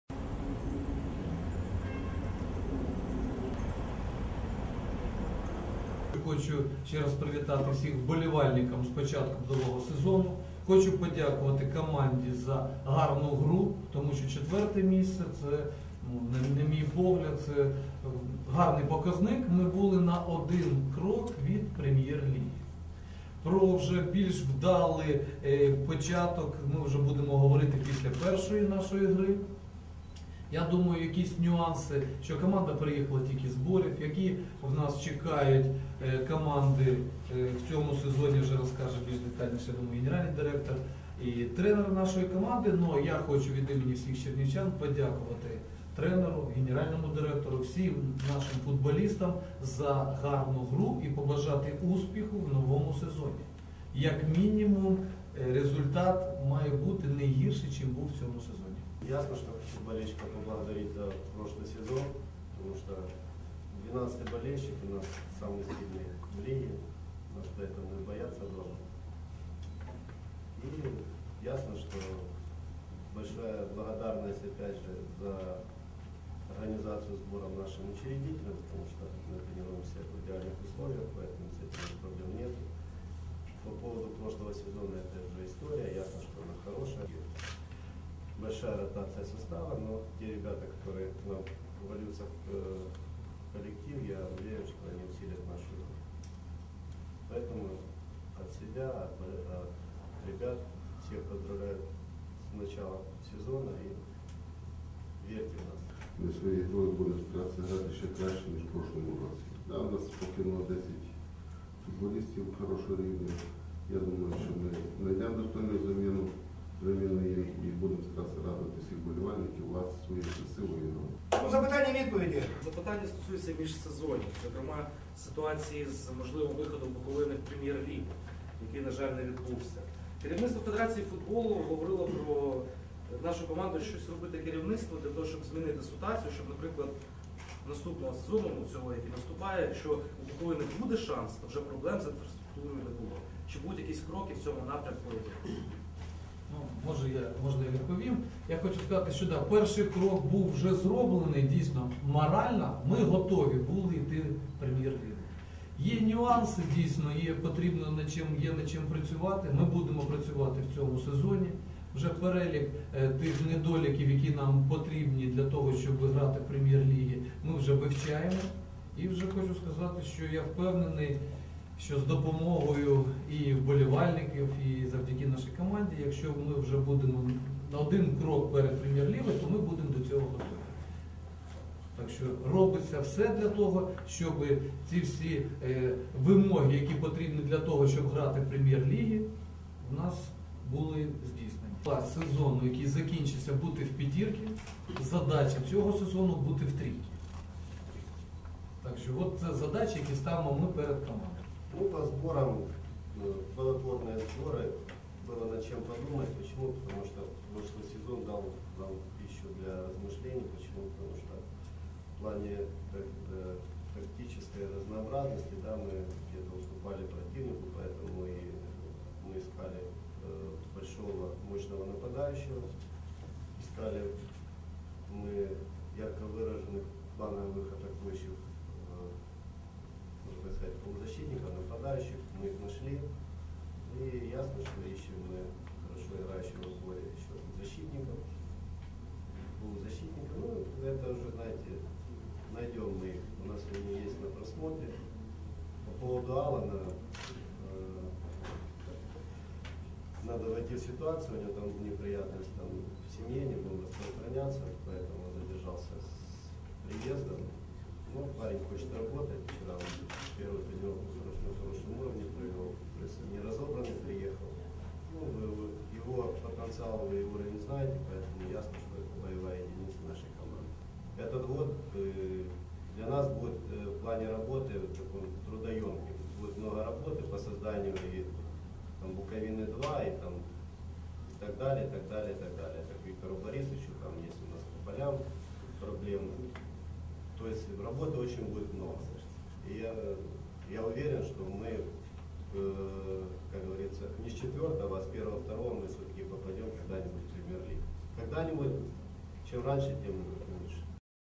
ФК «Буковина» представив нових гравців
Вчора, 12 липня,  футбольний клуб «Буковина» офіційно представив своїх новобранців у конференц-залі готелю «Буковина». Віталій Михайлішин, який є почесним президентом ФК «Буковина», вручив новачкам команди іменні футболки.
«Минулого сезону наша команда показала хорошу гру і посіла досить високе ІV місце у чемпіонаті. Зараз завдання мінімум - місце в трійці»,- заявив Віталій Михайлішин.

Стартовий матч першолігової першості України з футболу 2013/14 «Буковина» зіграє  вдома у неділю, 14 липня, проти краматорського «Авангарду».
Інтернет-канал «Чернівці-ТВ» традиційно здійснюватиме пряму трансляцію футбольного матчу.

Галина Єреміца